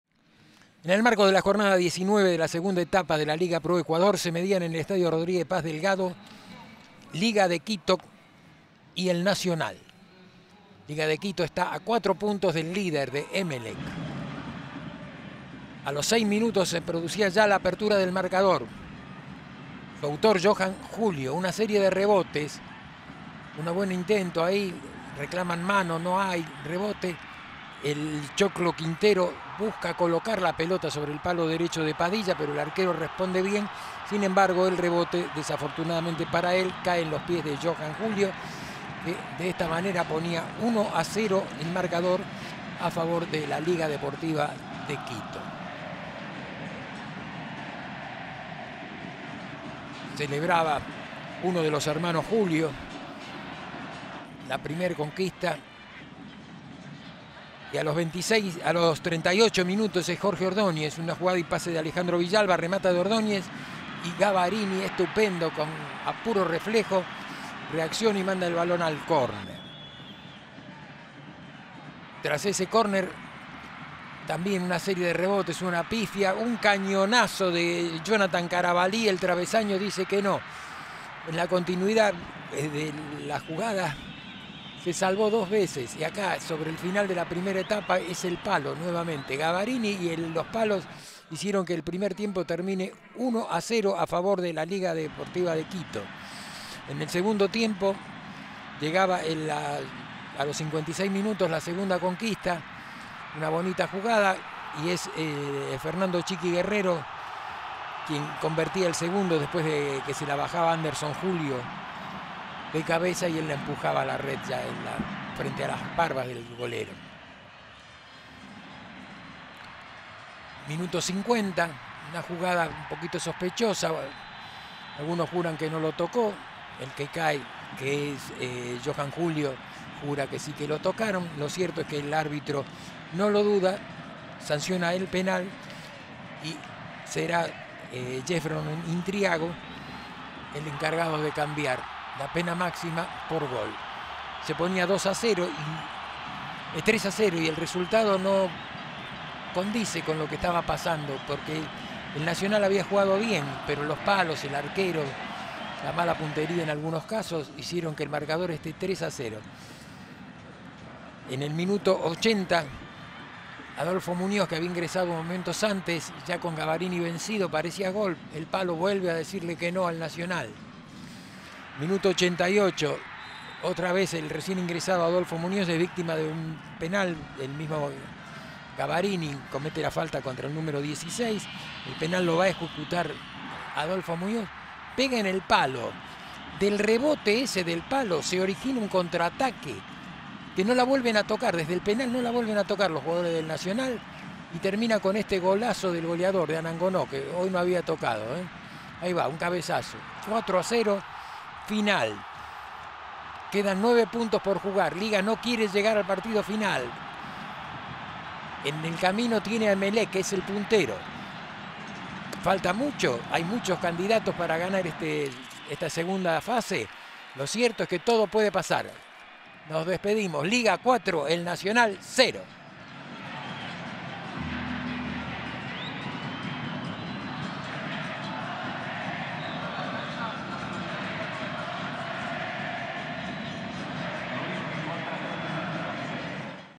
Liga 4:0 El Nacional
LDU QUITO 4 - 0 EL NACIONAL

Liga Deportiva Universitaria de Quito x CD El Nacional
Competición: Primera A
Fecha: 24 noviembre 2018 | Jornada 20 | Final: 4 - 0
Estadio de Liga Deportiva Universitaria (Quito, Ecuador)

GOLES
1 - 0 | 6' J. Julio
2 - 0 | 56' F. Guerrero
3 - 0 | 60' J. Intriago (Penal)
4 - 0 | 89' J. Anangonó

GOLTV. This is Football. Esto es fútbol. ⠀